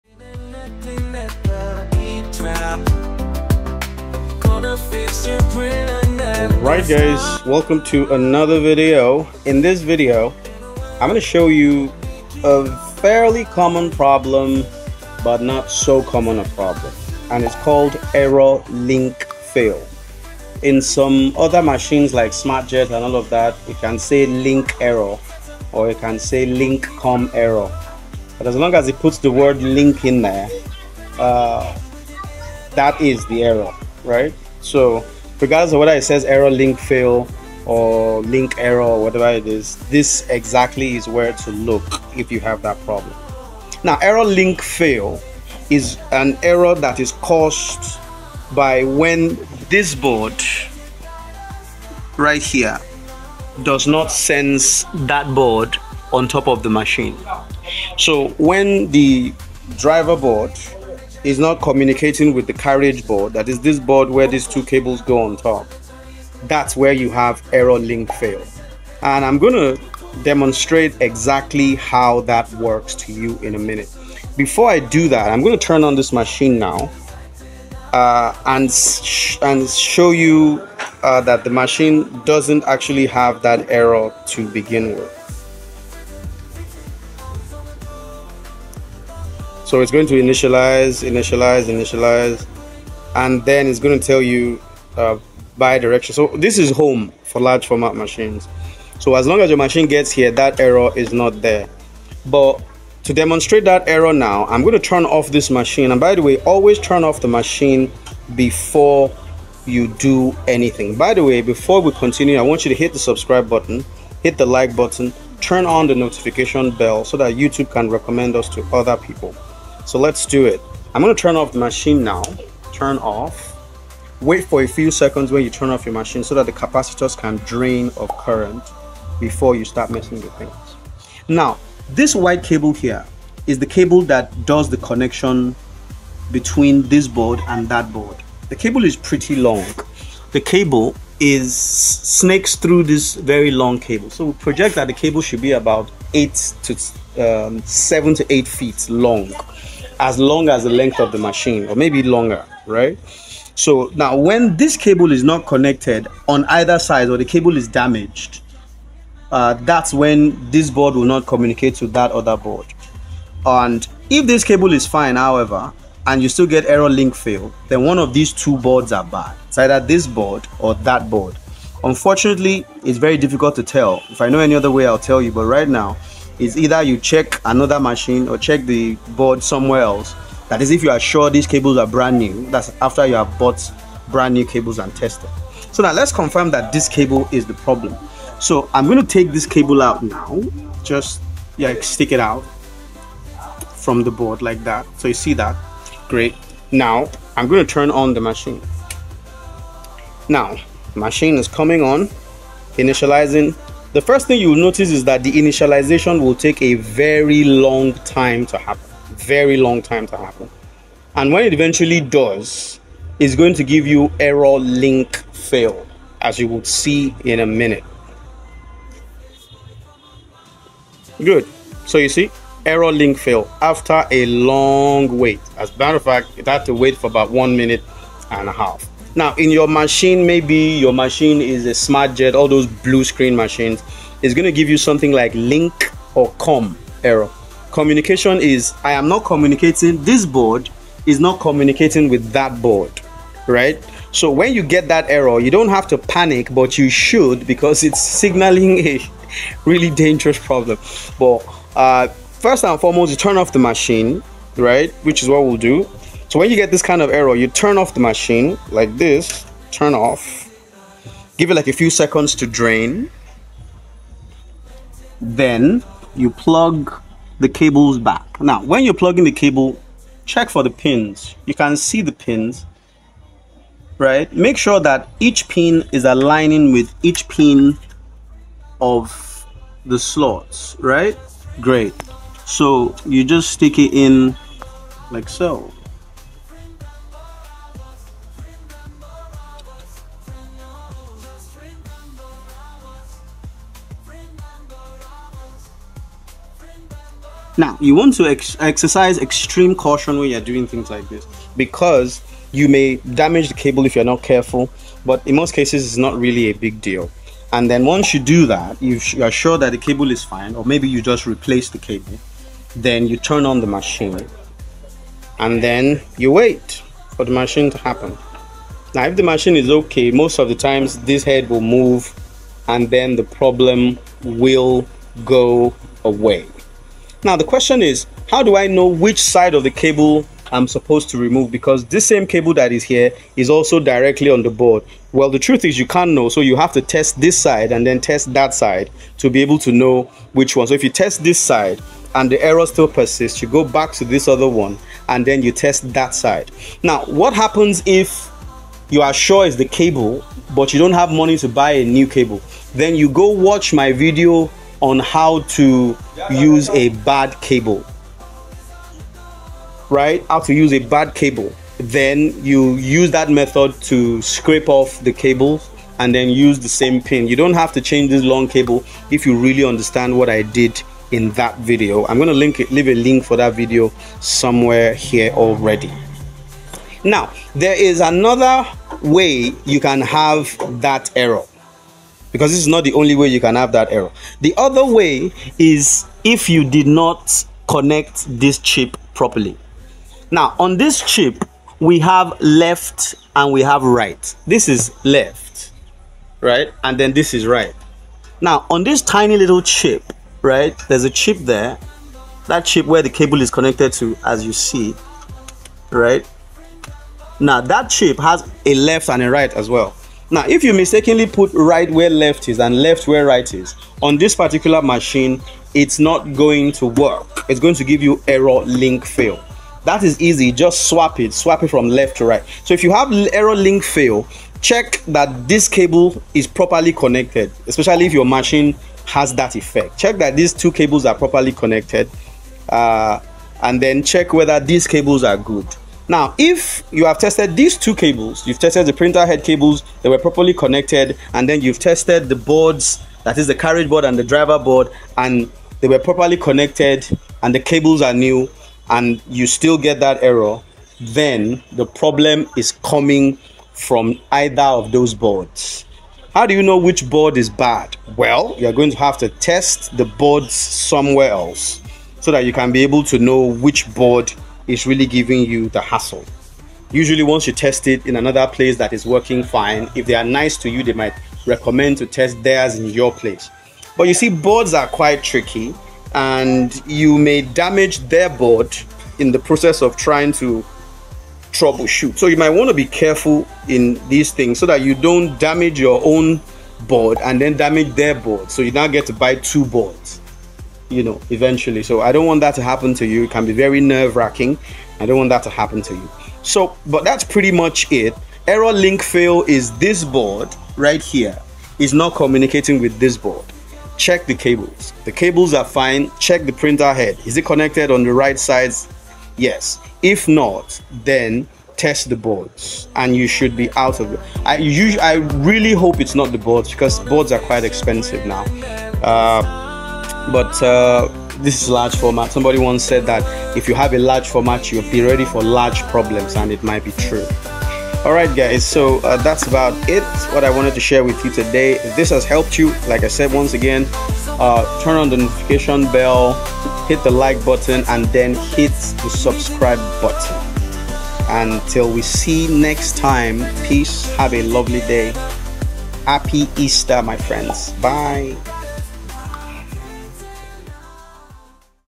ERROR LINK FAIL..??? WATCH THIS TO SOLVE THE PROBLEM
So let's talk about my new experiences with the yinghe XP600 large format.

So let's say you run into this error (see photo in post) Well there are three common causes of this problem.

If you use an old model yinghe XP600 LARGE FORMAT MACHINE its basically two.

One likely culprit is the cable the runs from the Driver Board all the way to the carriage board. See that cable. Is what links all the connectors of your machine to the print Job at hand.

In other words, that cable is essentially the link. Which is why the error say link fail.

If you run into this kind. Then the best bet is to change the cable. This is your cheapest route.

The other cause may be the carriage board itself. Because. Unless you have remove the long flex cable in recent past there is no reason to suspect the cable.

Then it just means you have a faulty board in your hands. Often at this point I feel bad because its as sad as it gets but then. Its your board.

The third cause however is for those that use the recent yinghe G series model. It turns out that of you misplace the cables of the printer head e.g if you plug the cable that comes from left side of the board to the right side of the head and vice versa.

Anyways that's it.

You can call.? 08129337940, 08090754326